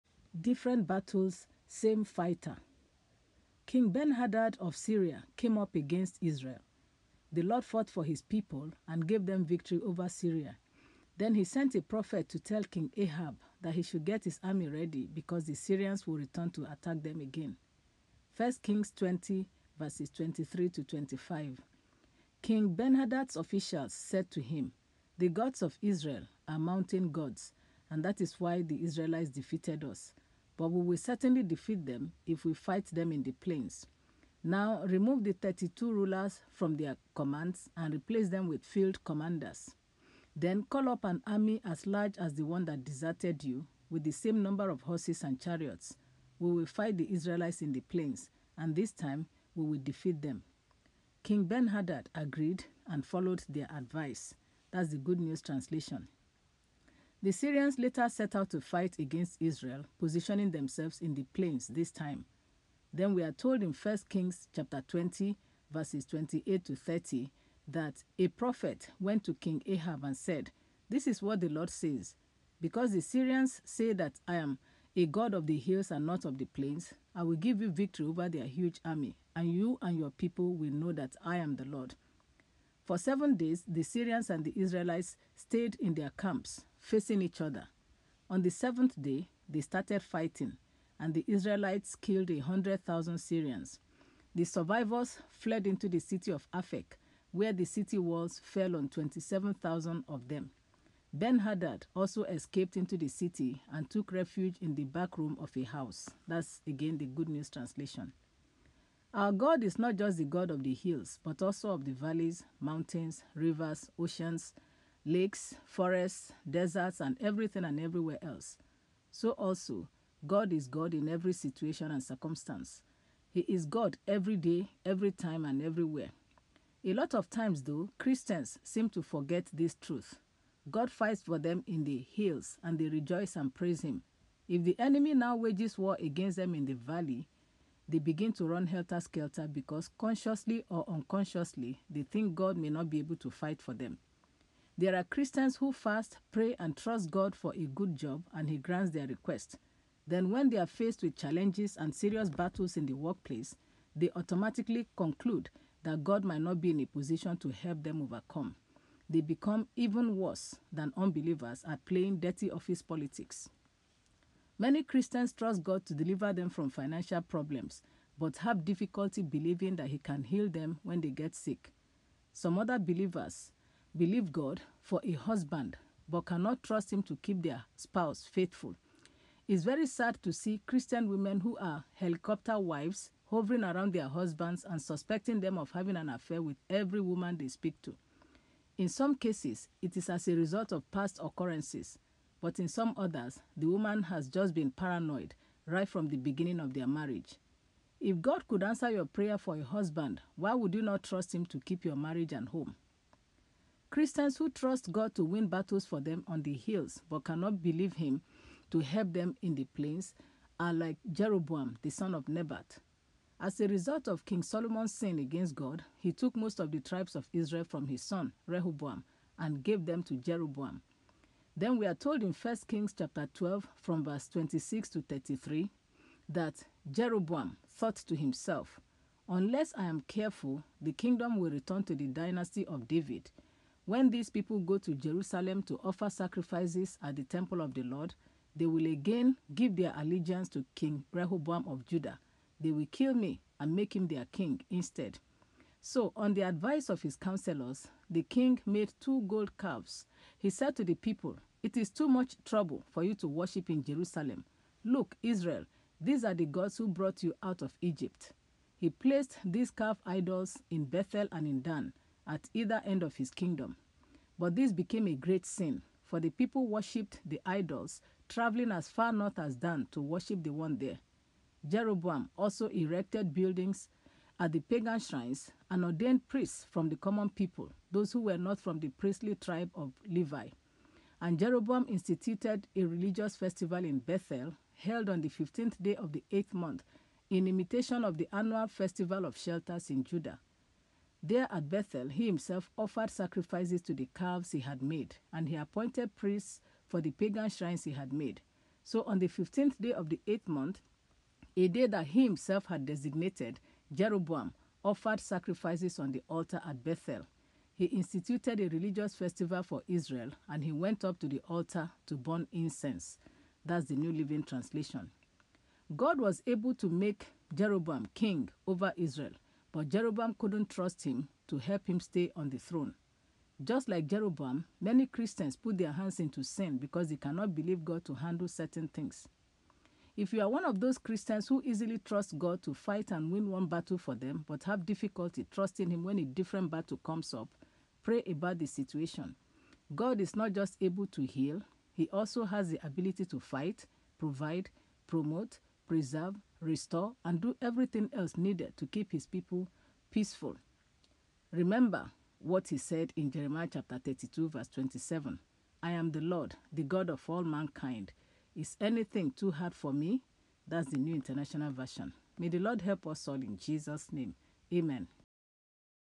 Different Battles, Same Fighter
God is not just able to heal, He also has the ability to fight, provide, promote, preserve, restore and do everything else needed to give His people peace. Remember what He said in Jeremiah 32:27, “I am the Lord, the God of all mankind. Is anything too hard for Me?” (NIV).